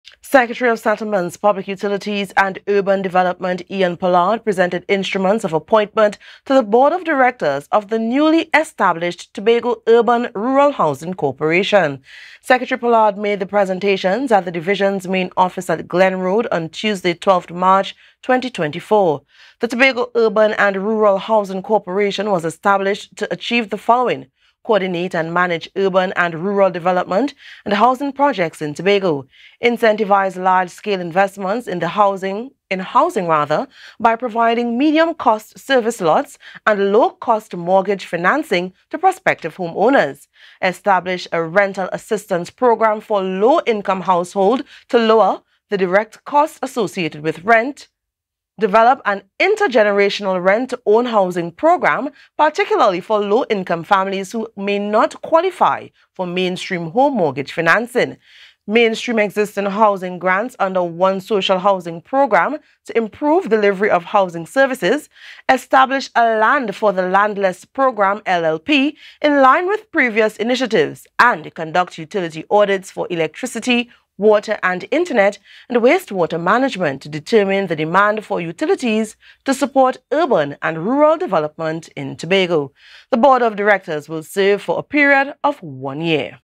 Tobago Urban Housing Corporation
Secretary of Settlements, Public Utilities and Rural Development lan Pollard presented instruments of appointment to the Board of Directors of the newly established Tobago Urban and Rural Housing Corporation.
Secretary Pollard made the presentations at the Division's Main Office at Glen Road on Tuesday 12th March 2024.